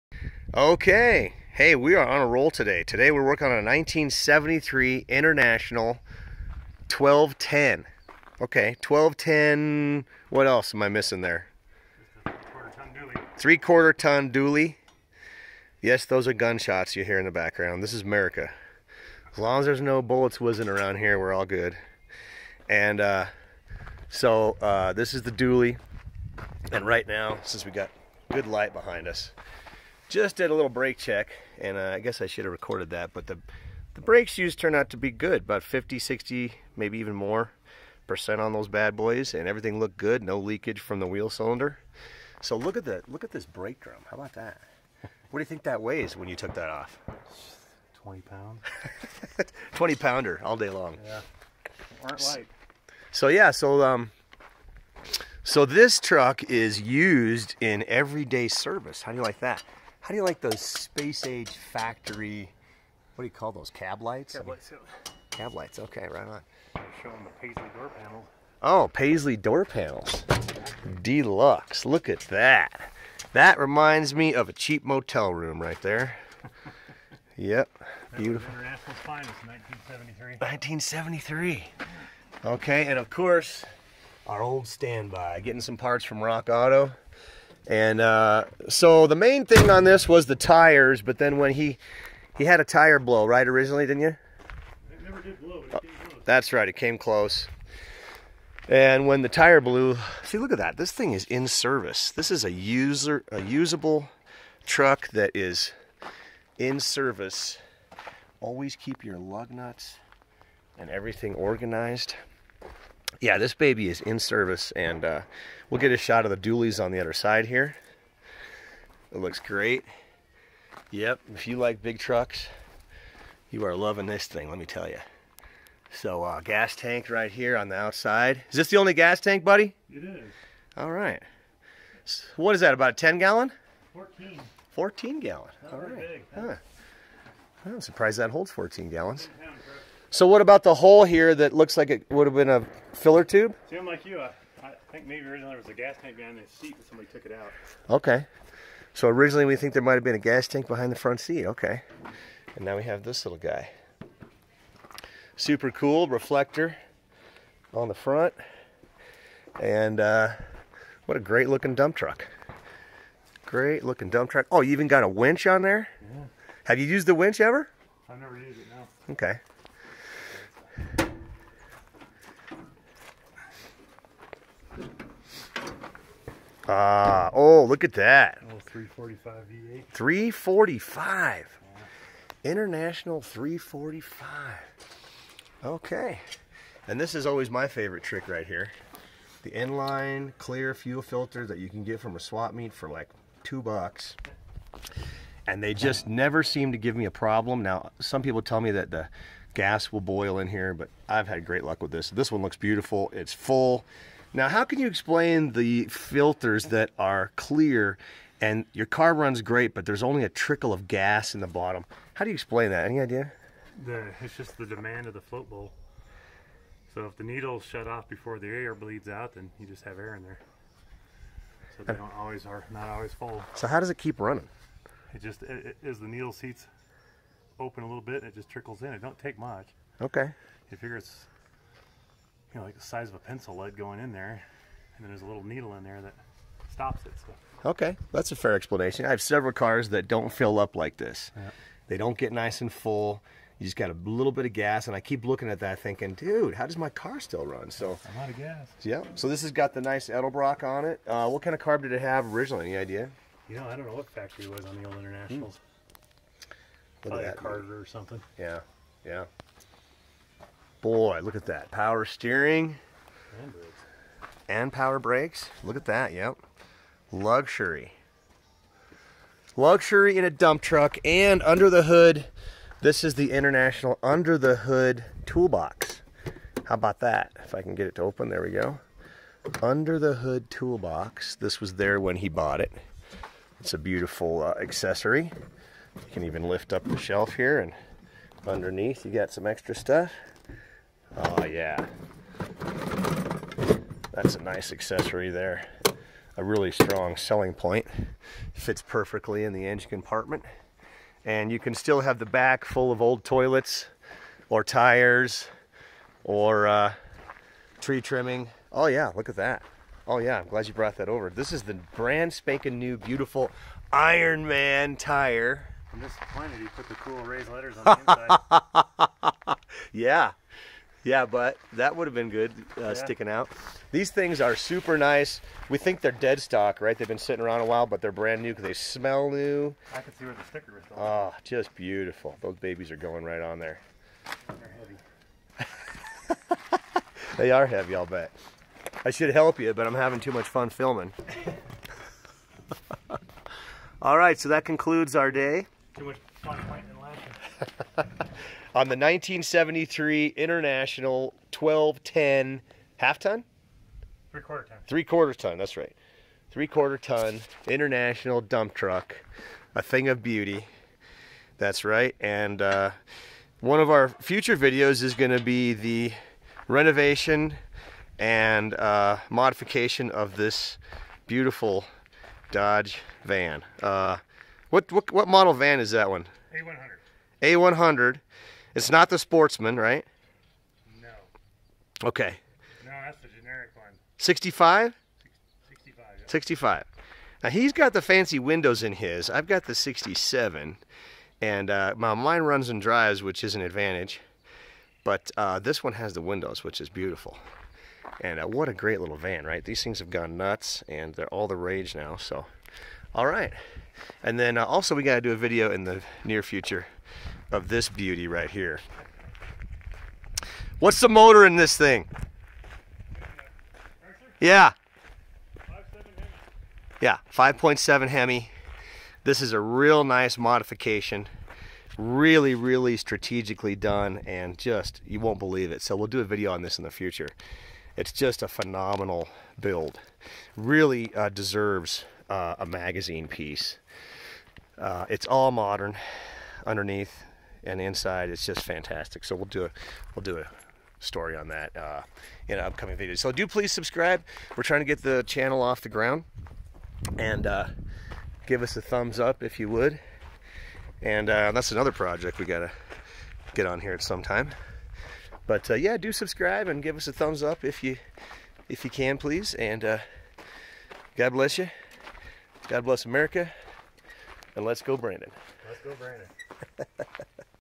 1973 international 1210, 3/4 ton, dually DUMP TRUCK! Factory 345 cubic inch V8
1973 international 3/4 ton 1210, dually dump truck! This thing is in service and gets used weekly! besides some new, dead stock tires, the Edelbrock carburetor was the only new upgrade I noticed. This thing is almost bone stock and earns its keep year-round! We put some new brakes on it and four new tires and fully intend to continue taking old toilets, construction debris, tree trimmings And whatever else will fit in the back of this beast to the dump (or my buddy Fish's front yard).
--let's go Brandon